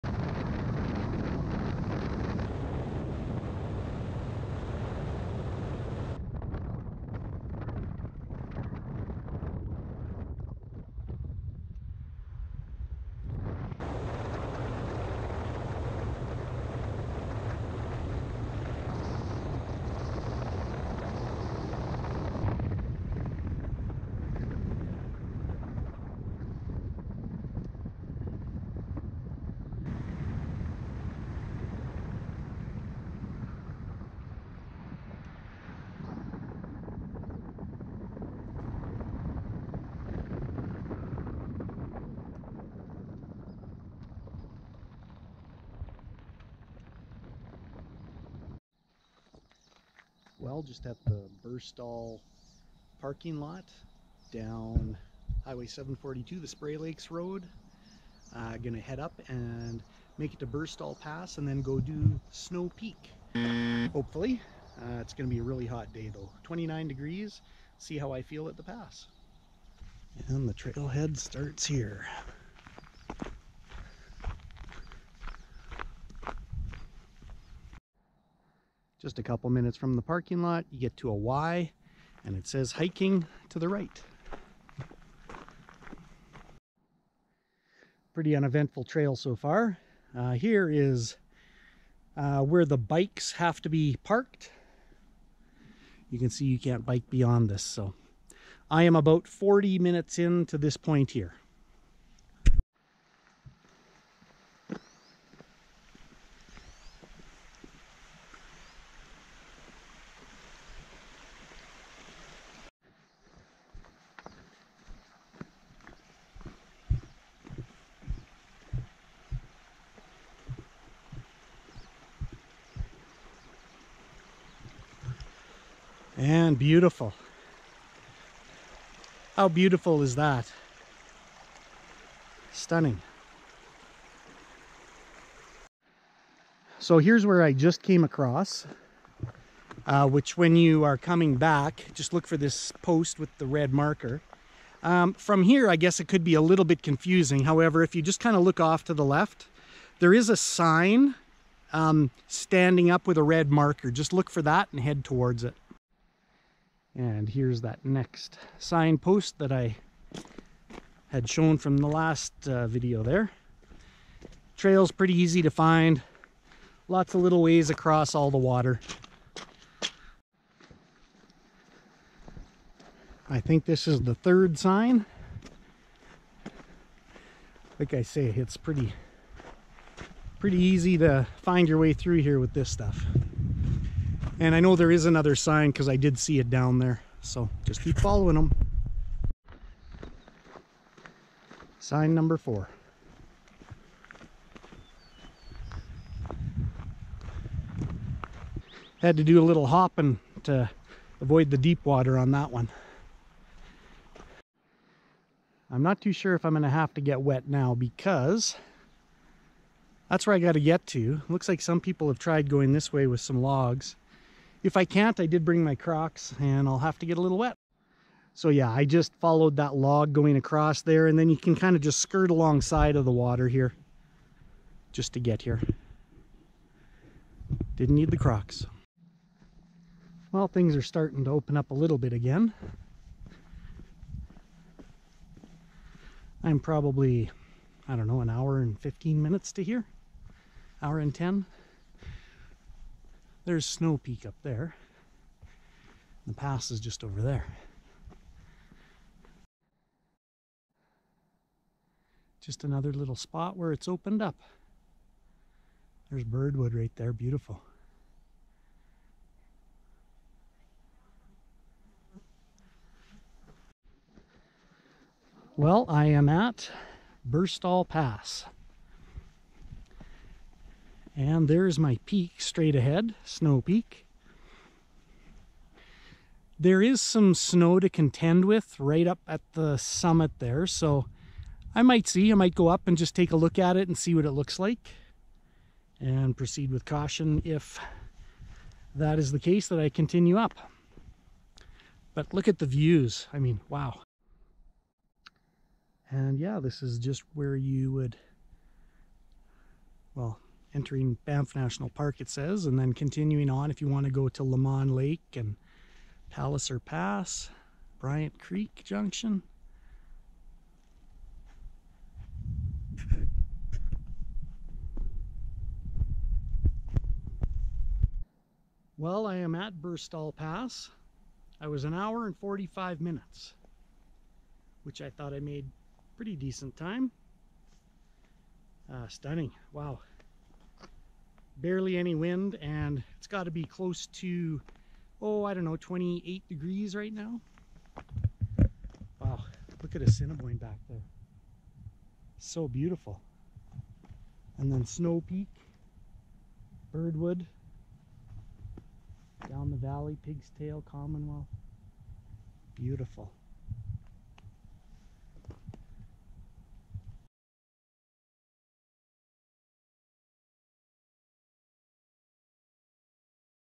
BURSTALL PASS hike, Peter Lougheed PP, Kananaskis (4K)
Roughly this hike is 15kms round trip with an elevation gain of around 480 meters. The first 3.0 kms can be biked to shorten the trip should you choose.  There is a point where you may need to put on sandals or crocs however I always found a way around the water in the meadows.

A beautiful day today for sure. +28 degrees and no wind. A little too warm but a perfect day for views. This hike was intended as an approach to ascend SNOW PEAK however there was a substantial amount of snow on the ridge at the top of Snow Peak which can be seen from the pass. Being that I didn't bring crampons or an ice axe I opted to just sit and enjoy the views at Burstall Pass. Didn't want to have an involuntary glissade. Enjoy.